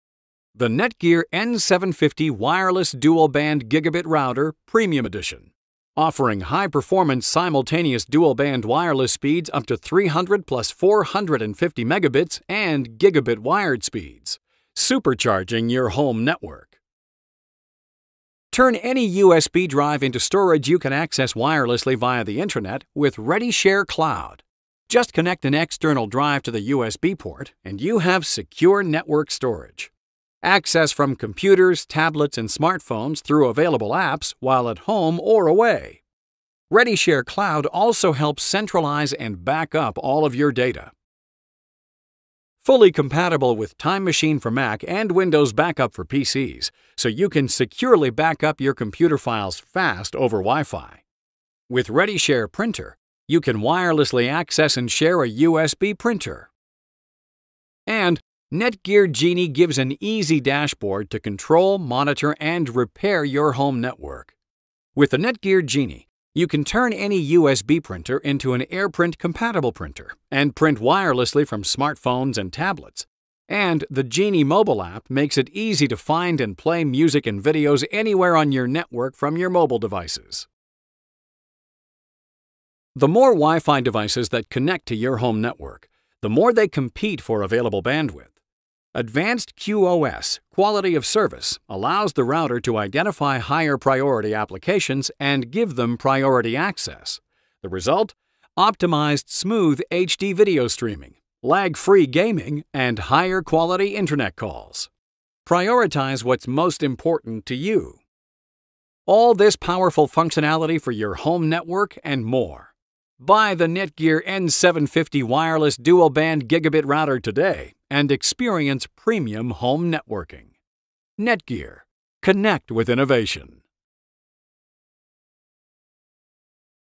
NETGEAR WNDR4300 Router Introduction
An overview of the NETGEAR WNDR4300 N750 Simultaneous Dual-Band Wireless-N Broadband Router (750Mbps)

NETGEAR WNDR4300 Key Features:

- Faster WIFI and Range:Up to 300 + 450 Mbps, improved for larger homes.
- Simultaneous Dual-Band: Reduces WiFi interference & doubles bandwidth.
- Secure WiFi Connections: Highest level wireless security with WPA/WPA2.
- Gigabit Wired: Ideal for HD gaming & video.
- DLNA: Find & play your media on DLNA TVs and game consoles.
- Readyshare USB Access: Wirelessly access and share USB hard drives.
- Readyshare Printer: Wirelessly access and share share a USB printer.
- Time Machine Compatible: Automatic Mac backup to connected USB hard drive, wirelessly.
- ReadyShare Cloud: Access and share files on a USB hard drive remotely.
- Parental Controls: Web filtering for all your connected devices.